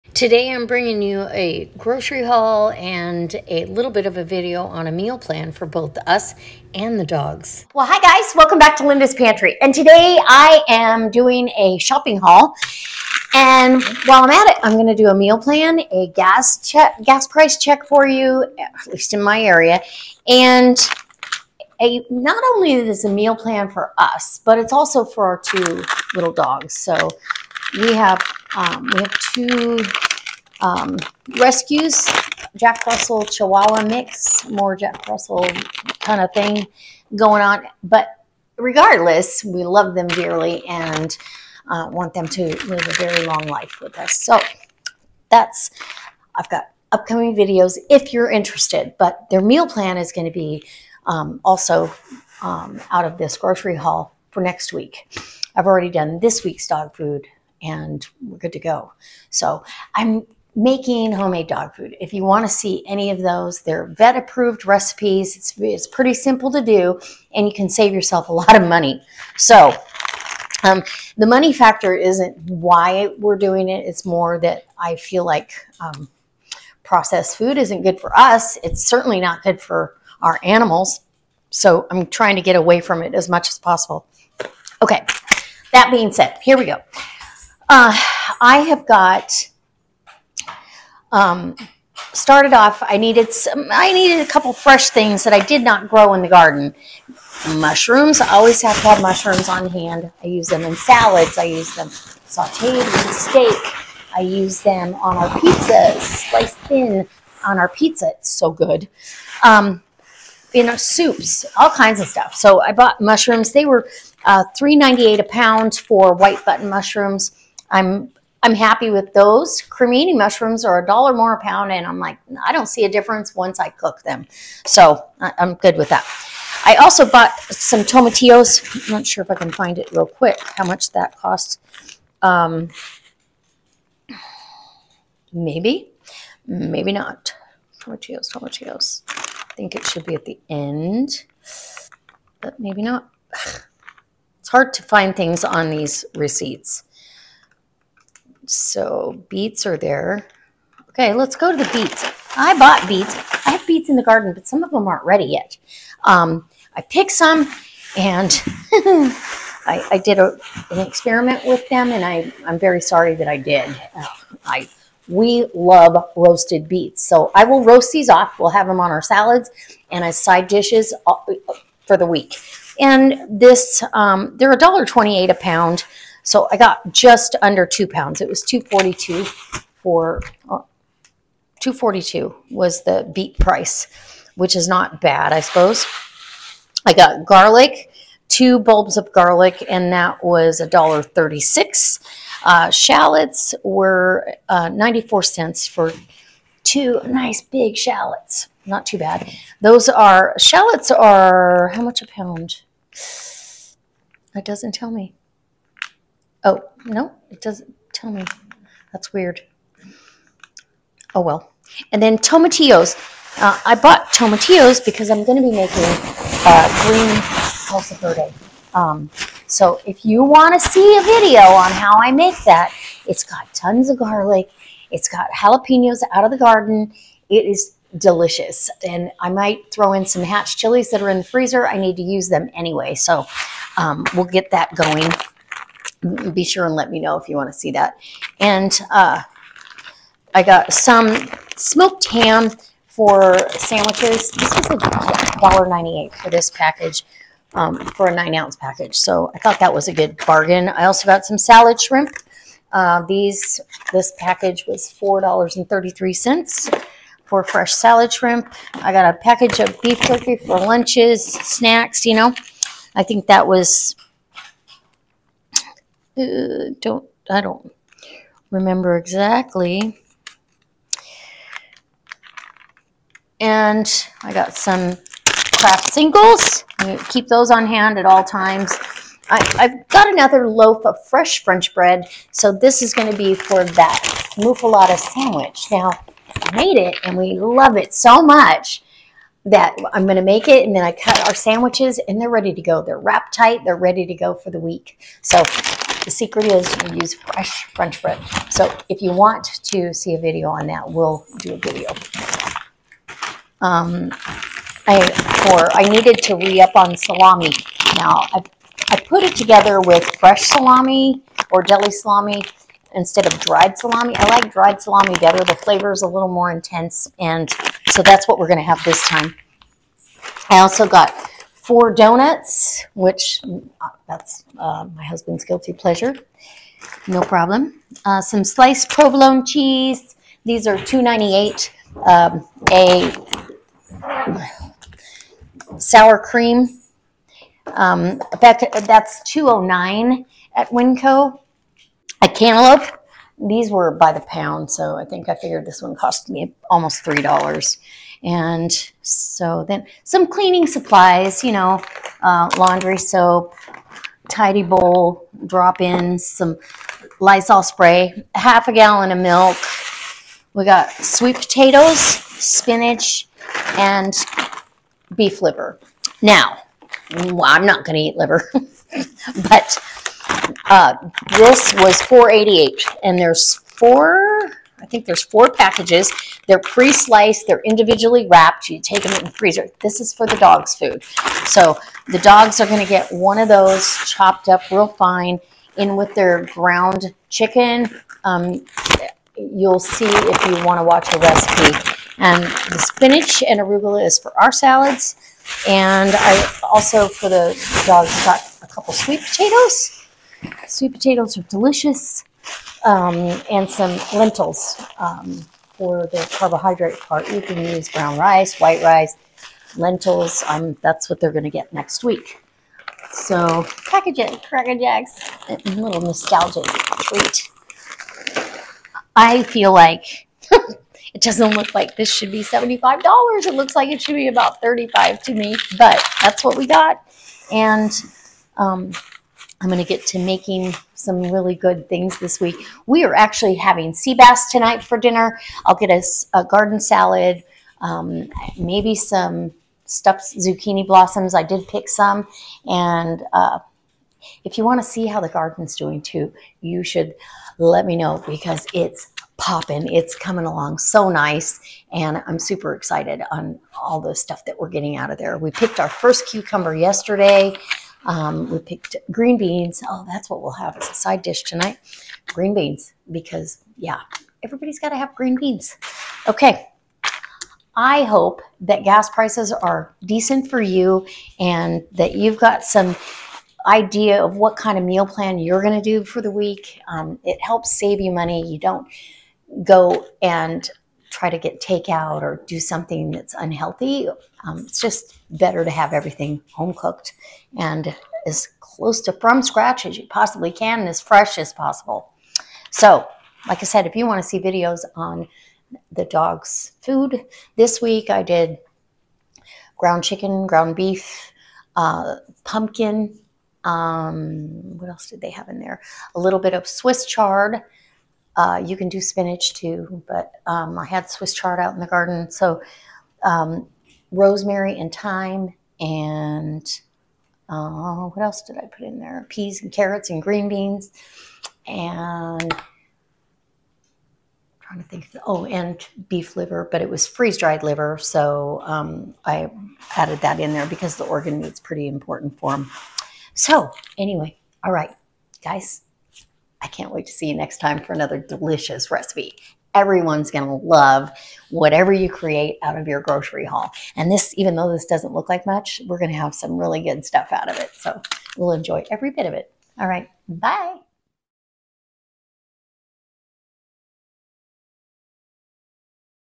Crazy Grocery Haul & Meal Plans For The Dogs And Us
Shopping haul that include free food from the garden to supplement our grocery bill and keep the cost down continuing this process into the next year of edging my way to not buying produce at all.